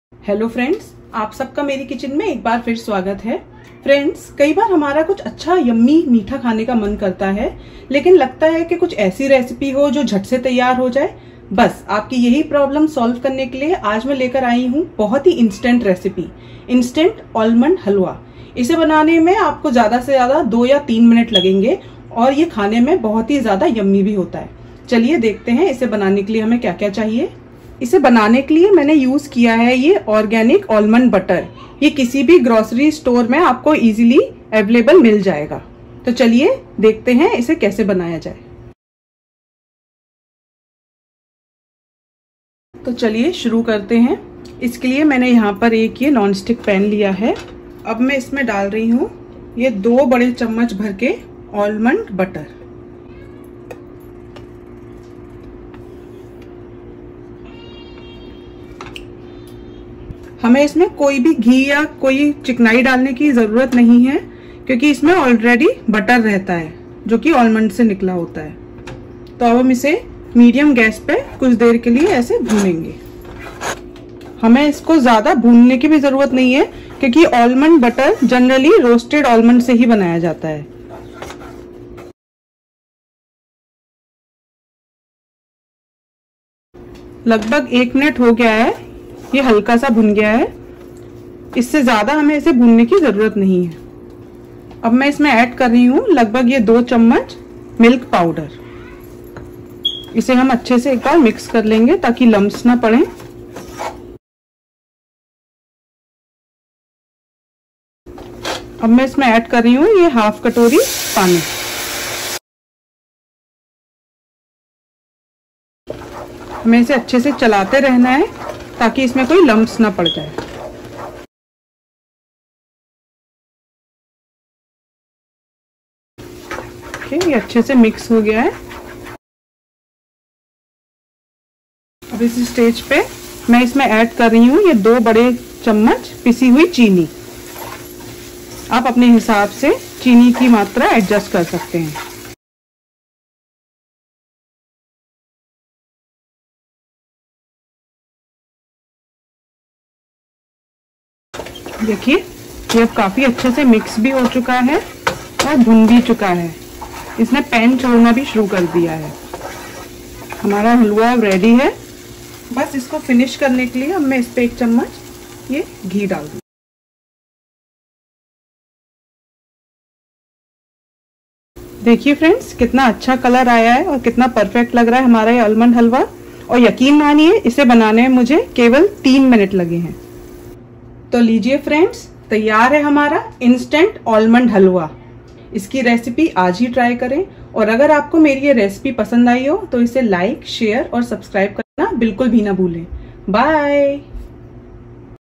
Winter special ....Instant almond halwa in just 2 minutes
Ingredients for Almond Halwa

2 Spoon Almond Butter
2 Spoon Milk Powder
Half Cup Water
2 Spoon Sugar
1 Small Spoon Ghee